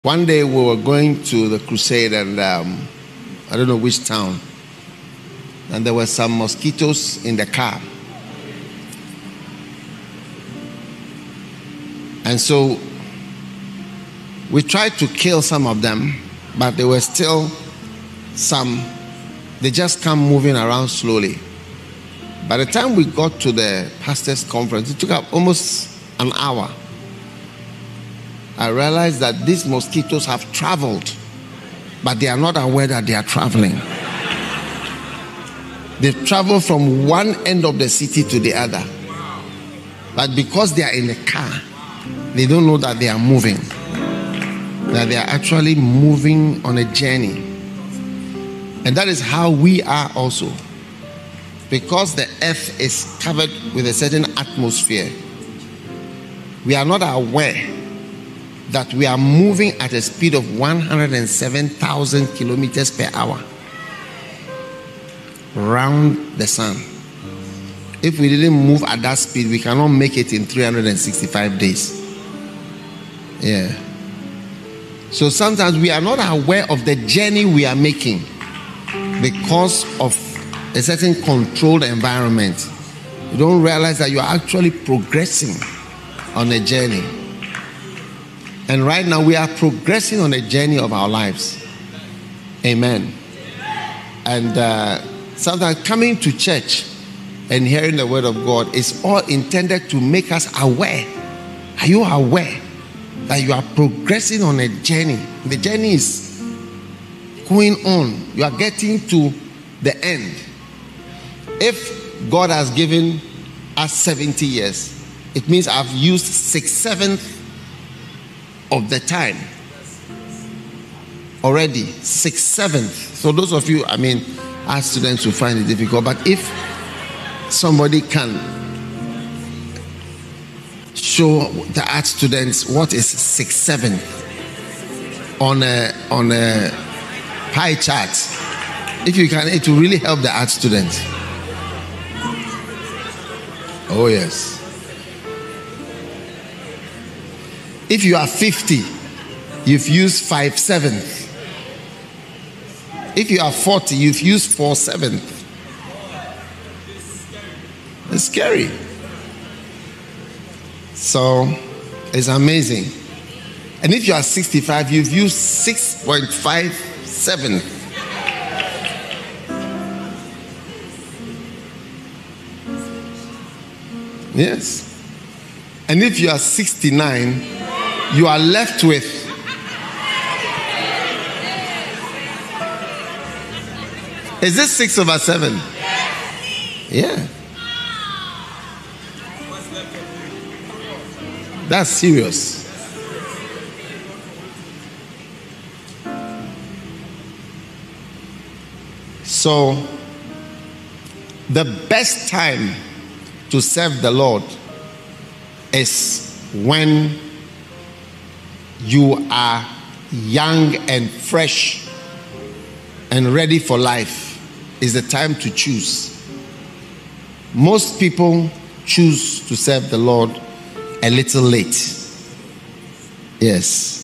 How many more years do I have on earth?_18th June 2023
The First Love Experience @ The First Love Centre, Accra - Ghana.
Prophet's return @ 60 from Madagascar and Gabon